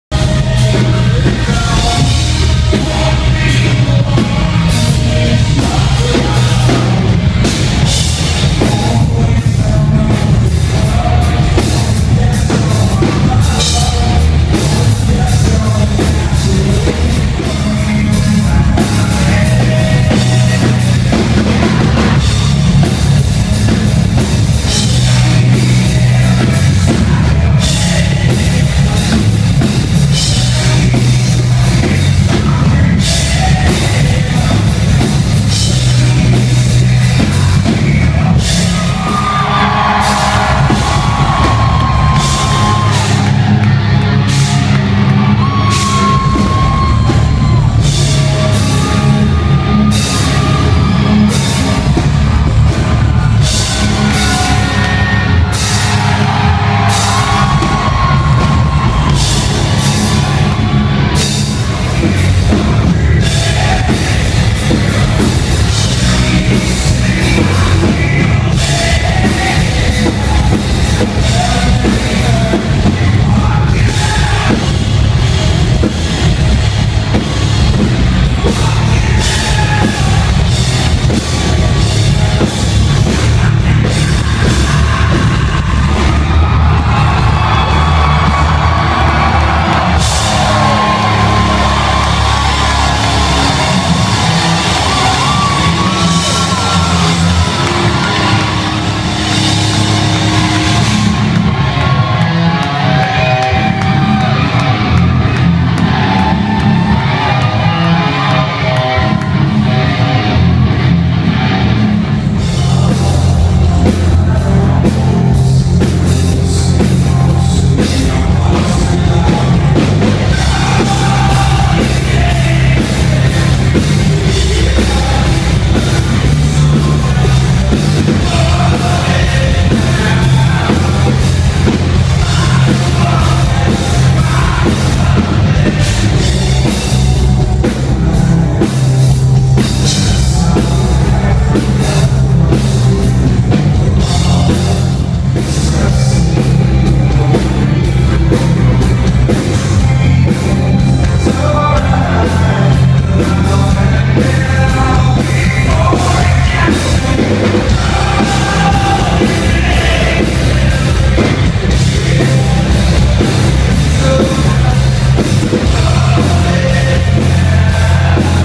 Seether Nashville war memorial auditorium
Side stage at the Nashville Seether show. Thx Jason